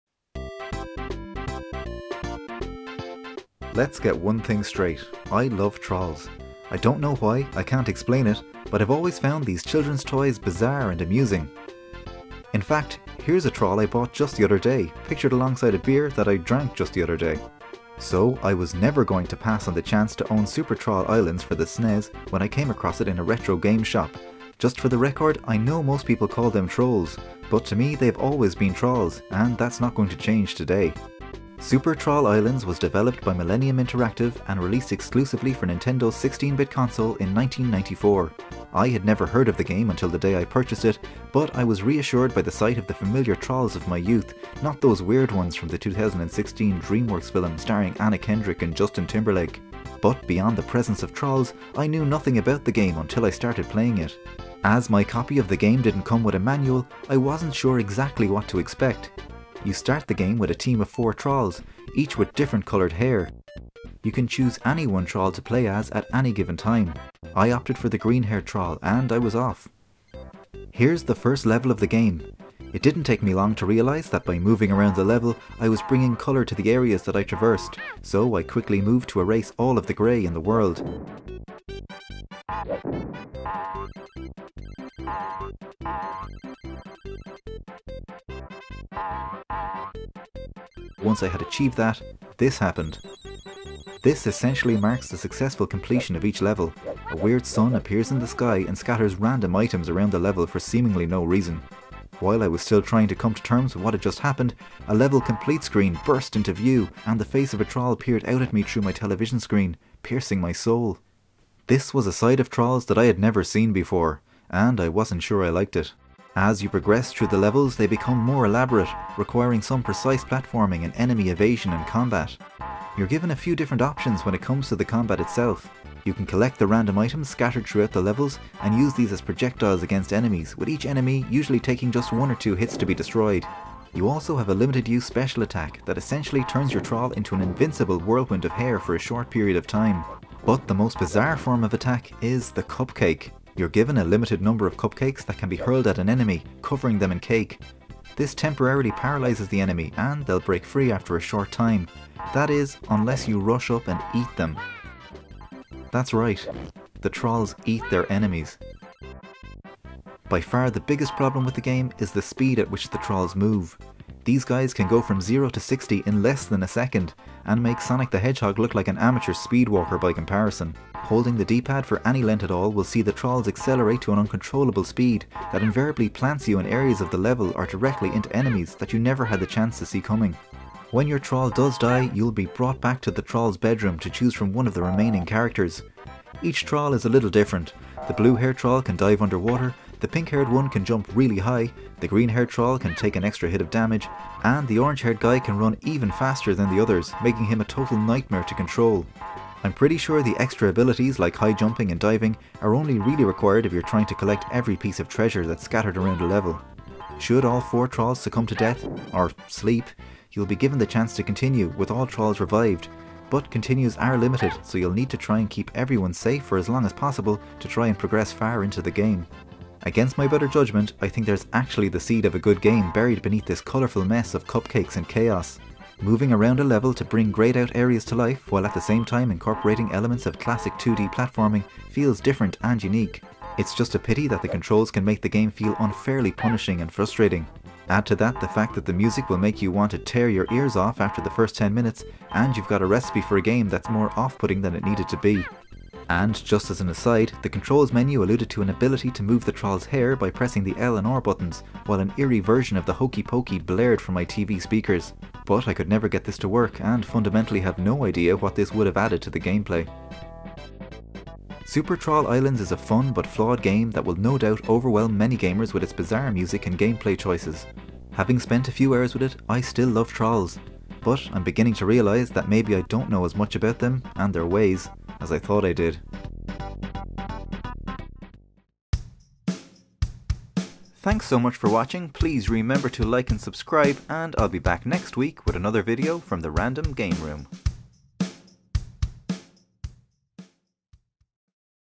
Super Troll Islands (SNES) - Review - Dave's Random Game Room
The trolls are on a mission to bring a little colour to the world in this bizarre platformer. They've got cupcakes and they know how to use them.

But is Super Troll Islands for SNES actually any good??

Here's another review from the Random Game Room!